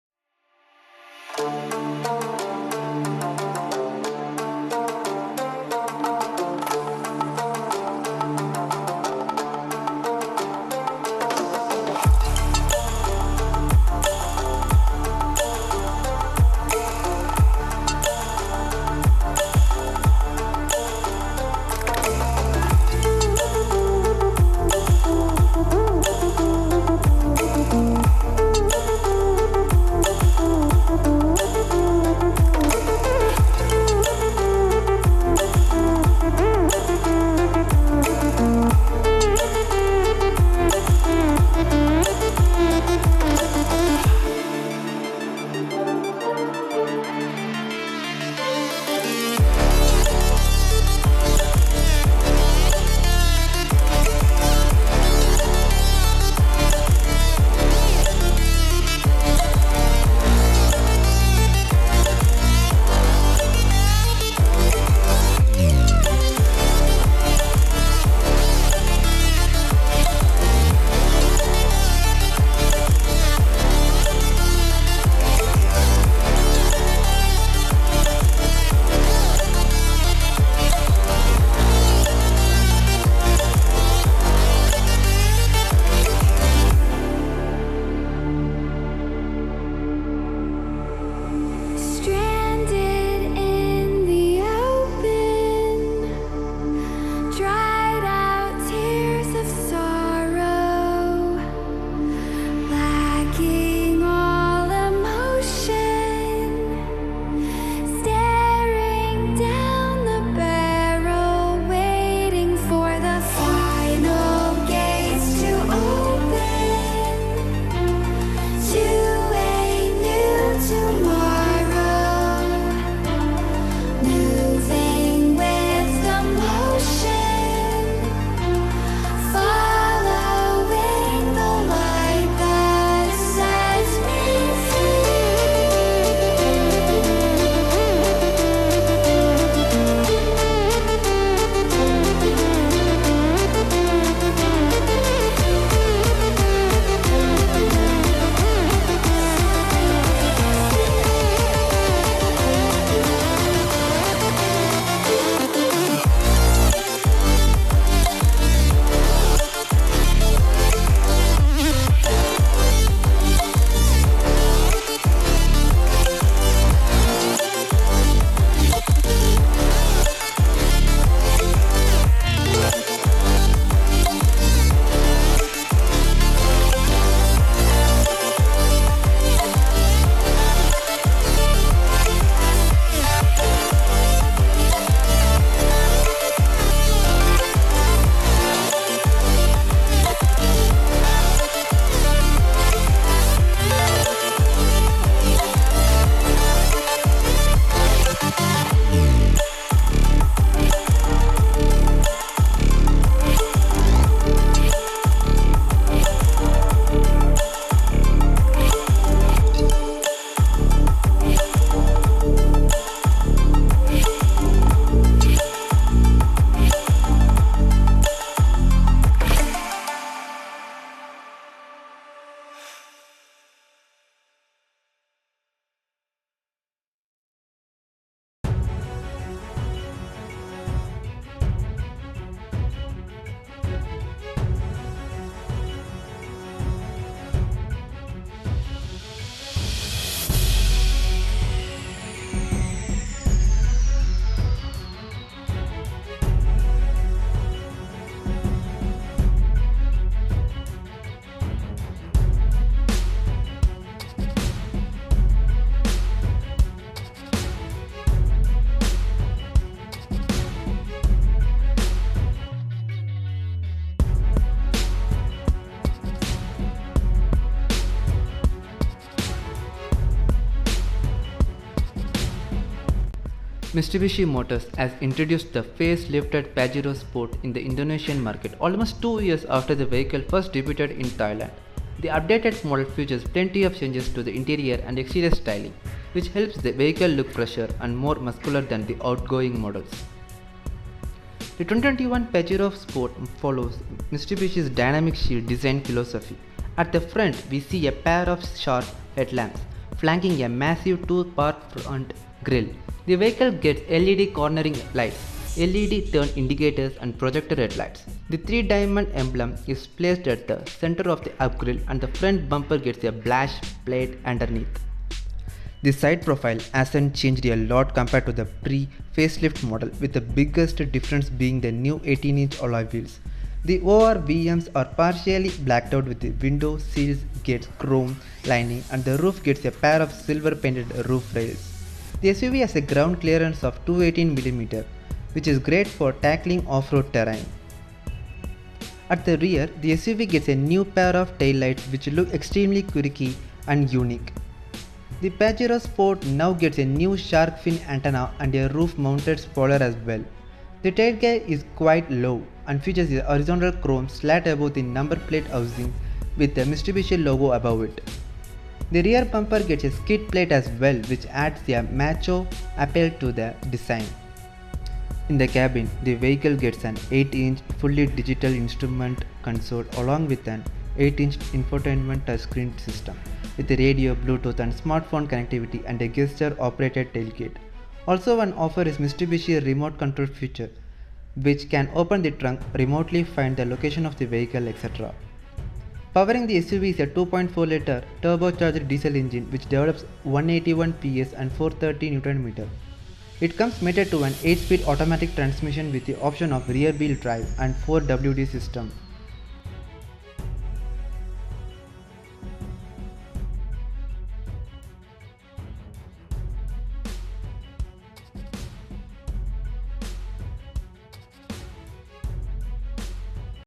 New 2021 Mitsubishi Pajero Sport Facelift - Family SUV Interior & Exterior
MITSUBISHI MOTORS CORPORATION (MMC) continues to expand its product portfolio by launching the refreshed PAJERO SPORT1 mid-size SUV in Indonesia. Inheriting the PAJERO series' off-road capability, reliability and durability, the PAJERO SPORT fuses elegance and comfort. The latest model is designed with a more refined, powerful exterior and high-quality interior, as well as newly adopted features such as an 8-inch colour LCD meter and Power Tailgate that allows users to open and close the tailgate with a smartphone.

The new Pajero Sport in 2021 brings several design changes on the exterior. Highlights of the updates are new dynamic shield grille design, power tailgate (with kick sensor and remote control, active cornering lamp, sunroof, new front combination lamp.

In addition, the exterior also offers a classy and high-quality design, with updates to the new better hood visibility, new 18-inch two-tone alloy wheels, new fin-type antenna and a new design of rear combination lamp.

Turning to the interior, there are also updates to: New Head Unit 8 "with smartphone-link display audio (sda), new lcd meter cluster 8", multi around view monitor, new design dual-zone climate control ac, new auto-dimming rearview mirror, new hand free steering switch, new floor console, and new ac power outlet.

From the side of the diesel engine, this car features a 4N15 2.4L VGT-Intercooler which produces the maximum output: 181 PS at 3,500 RPM, 43.8 Kgm of torque at 2,500 RPM. Supported by an 8-speed automatic transmission with sequential sport, 218 mm ground clearance and a minimum turning radius of 5.6 meters. In addition, the New Pajero Sport is equipped with an adaptive cruise control feature. remote control connected with smartphone, super select 4wd-ii, off-road mode, and paddle shifters.

Meanwhile, safety features are equipped with forward collision mitigation (fcm), ultrasonic miss-acceleration mitigation system (ums), blind spot warning - lane change assist (bsw-lca), rear cross traffic alert (rcta), electric parking brake (epb). , brake auto hold (sump) 2. Not only that, security features such as 7 srs air bags, active stability & traction control (astc), anti-lock braking system, electronic brake distribution, brake assist, have also been pinned and supported by the technology rise (reinforced impact safety evolution) body, and rigid lightweight body.

Coming soon

Coming Soon